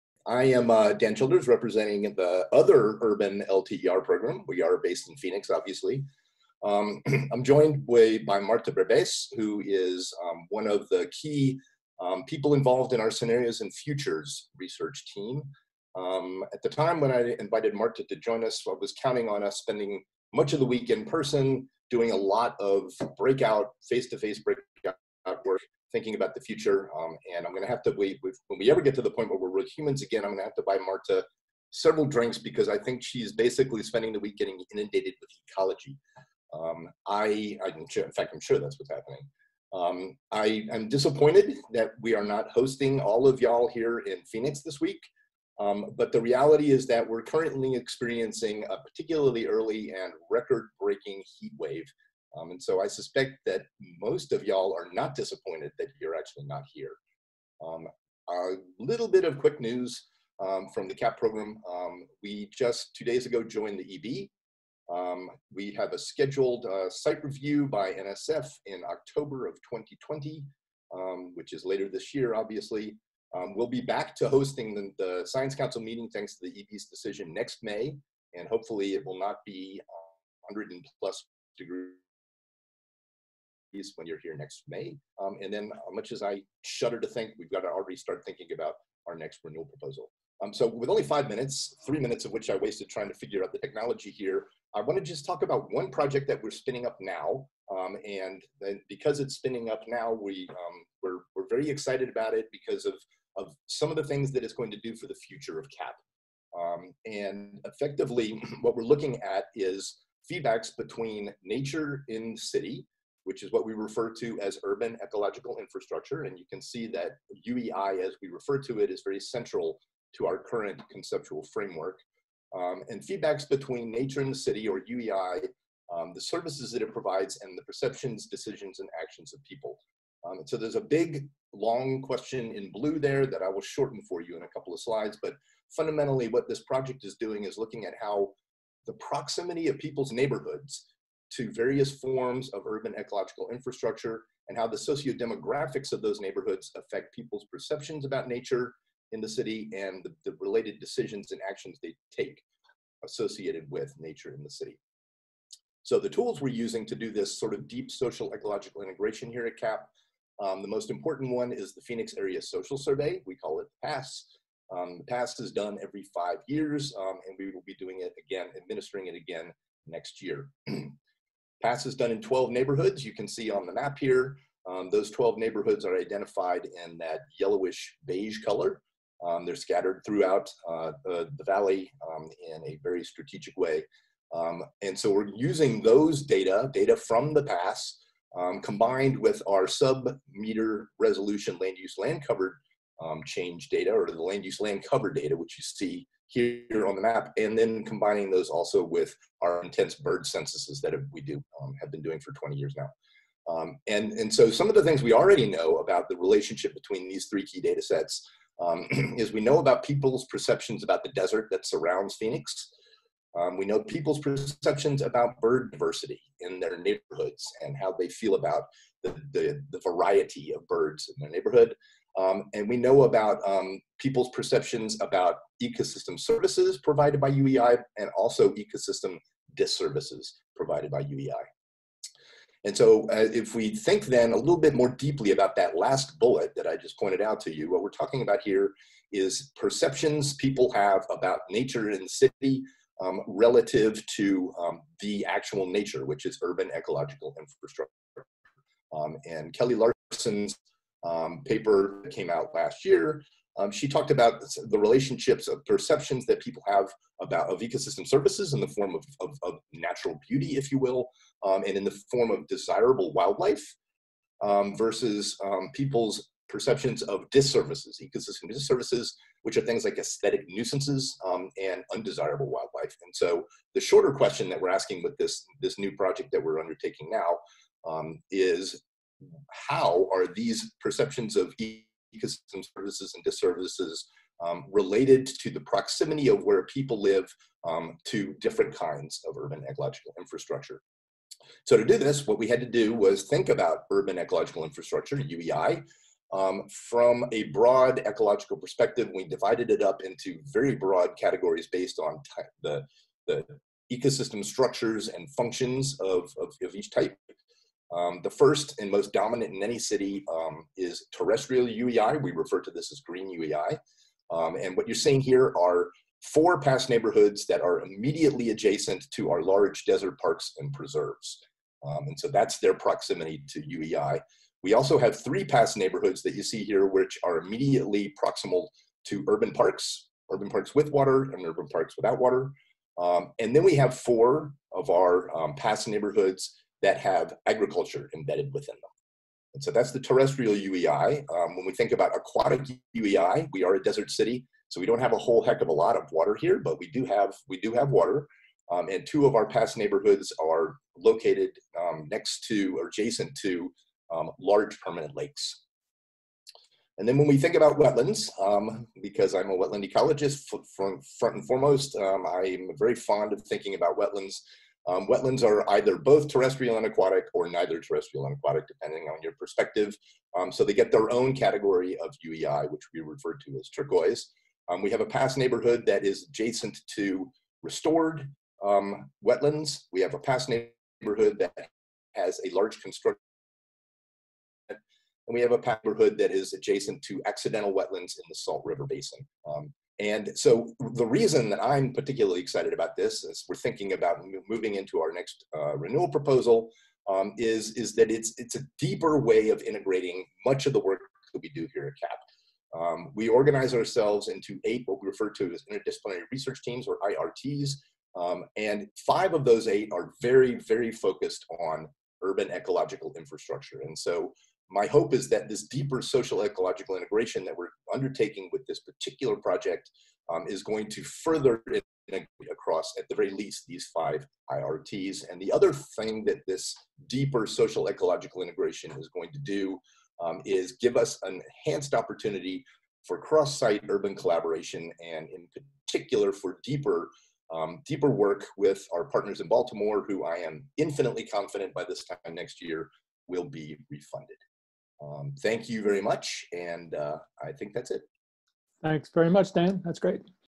Central Arizon-Phoenix LTER Site Lightning Talk 2020 05 06
Dan Childers, Principal Investigator of the Central Arizona-Phoenix LTER Program shares current research news and emerging directions for the site. Each year, investigators in the Long Term Ecological Network come together to share the most interesting science news from their sites. In 2020, due to COVID19-related physical distancing, these talks took place on line. Each talk is presented individually and viewers can also find discussion segments posted